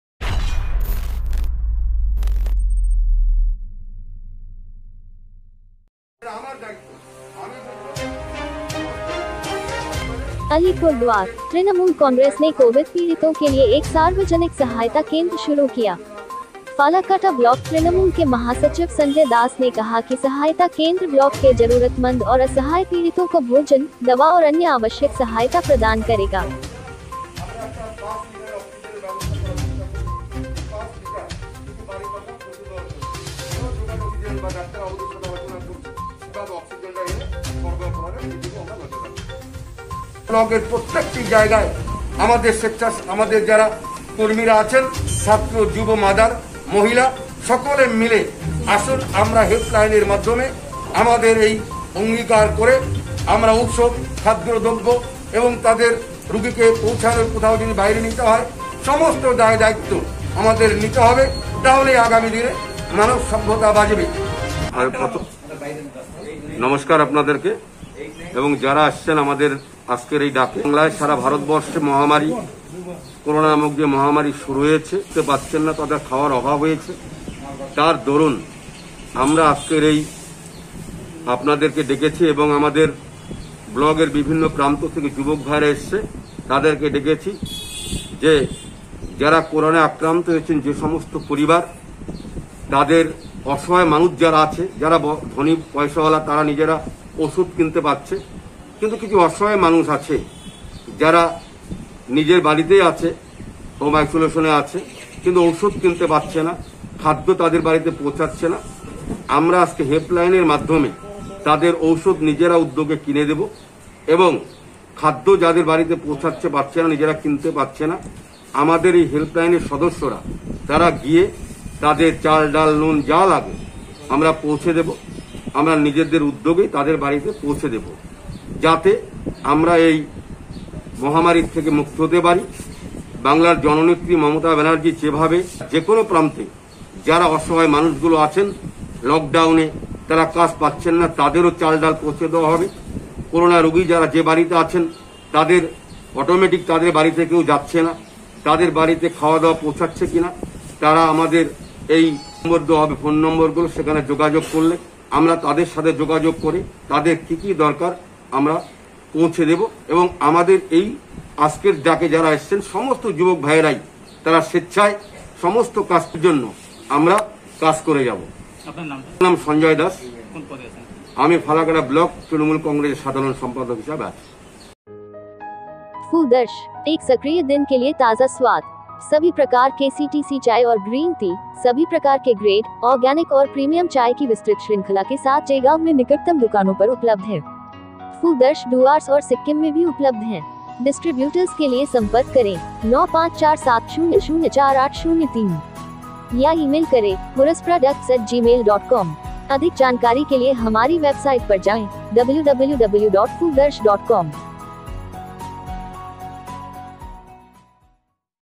अलीपुरद्वार: - तृणमूल कांग्रेस ने कोविद पीड़ितों के लिए एक सार्वजनिक सहायता केंद्र शुरू किया ।
फालाकाटा ब्लॉक तृणमूल के महासचिव संजय दास ने कहा कि सहायता केंद्र ब्लॉक के जरूरतमंद और असहाय पीड़ितों को भोजन, दवा और अन्य आवश्यक सहायता प्रदान करेगा।